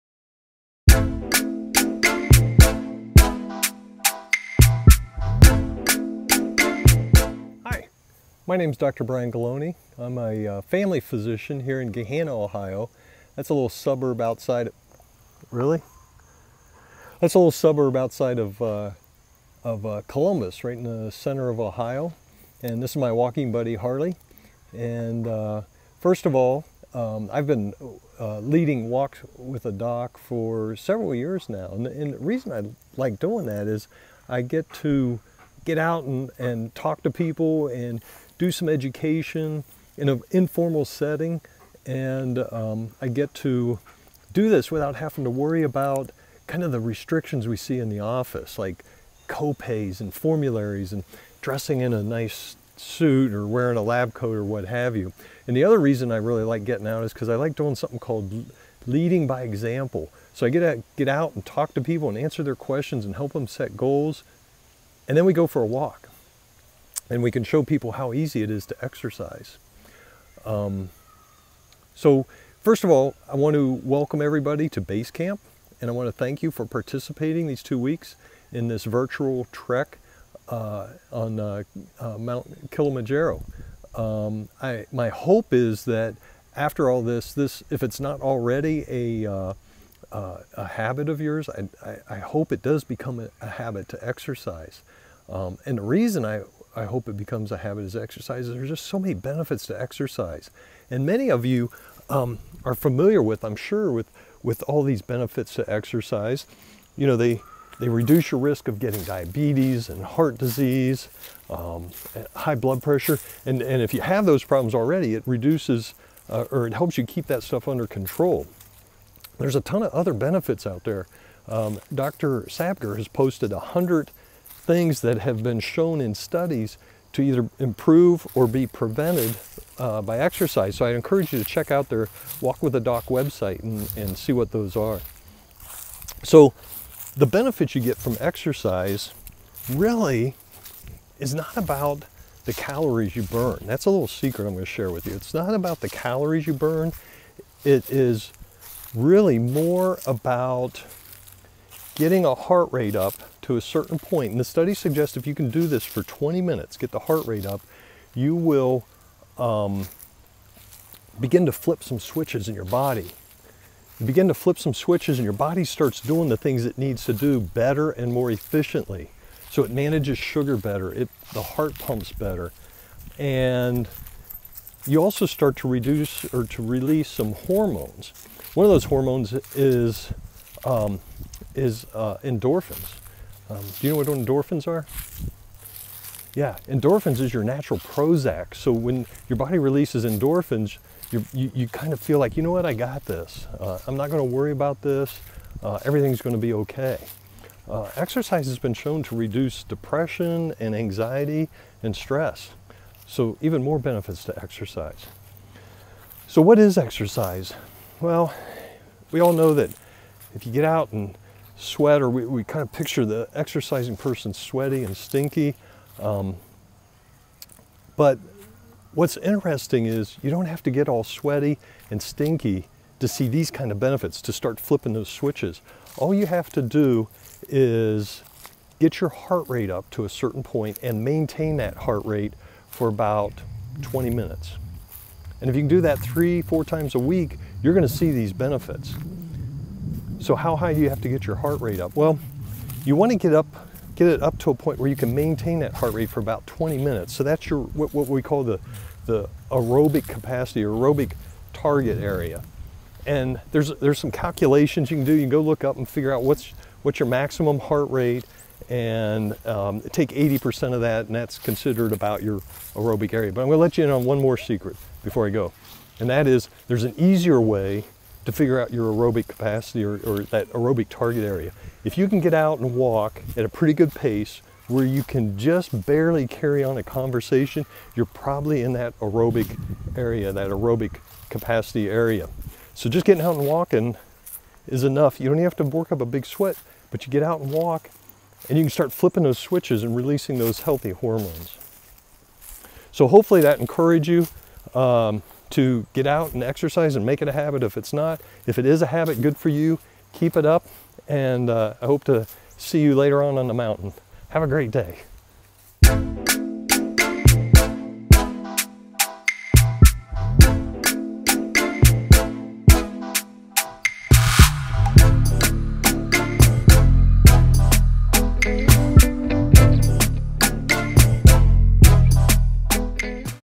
Basecamp: Dr. Ghiloni - Benefits of Exercise
In this Basecamp video for the 2020 Walk with a Doc Challenge, Dr. Bryan Ghiloni of Columbus, OH shares some of the many benefits of exercise.